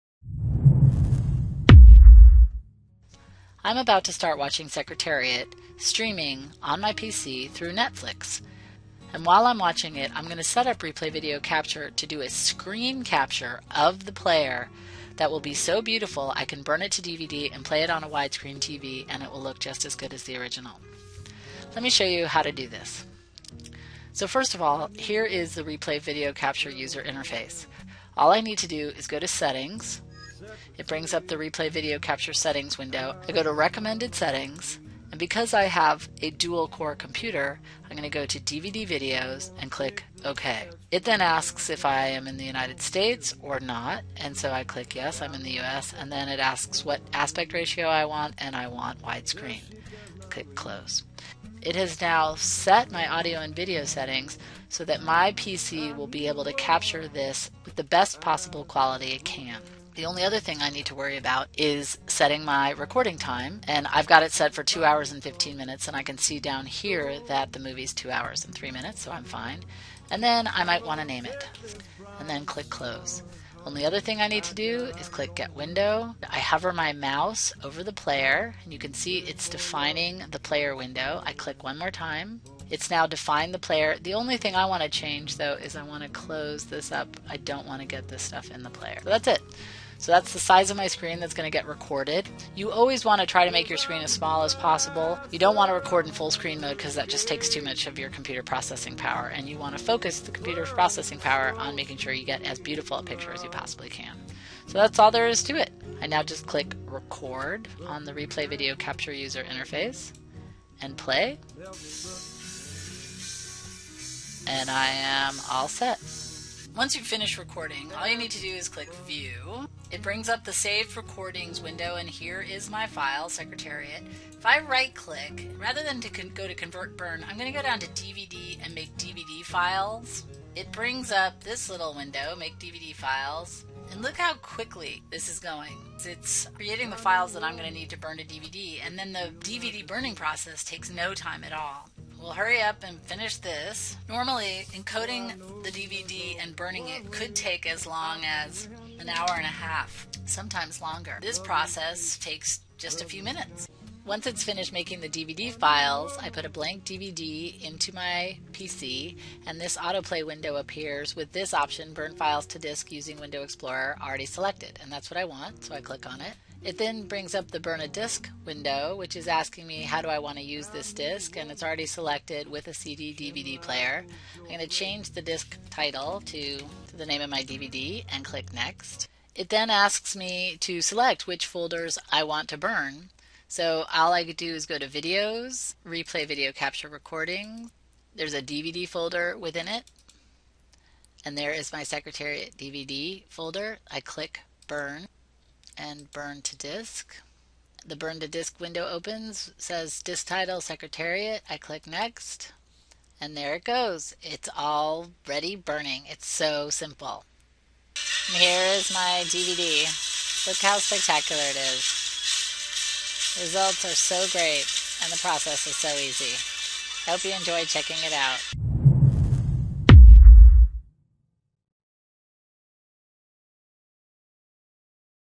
Burn a DVD from Netflix with Screen Recorder Software - Replay Video Capture
Burn a DVD of any Netflix movie using our professional screen recorder software.
Replay Video Capture 5 makes it super easy to capture and burn a DVD.

It's so easy:

1.) Replay Video Capture 5 has built in settings that maximize the quality of your captured recording for your specific PC set-up.  You are guaranteed a beautiful recording that will play on a wide-screen TV with professional quality.

2.) Replay Video Capture 5 offers dual monitor support - you can set up Replay Video Capture on one monitor while you work on another

3.) Replay Video Capture 5 offers the ability to make DVD ready files of your recordings in minutes - burning is so fast and easy.

Try burning a DVD today.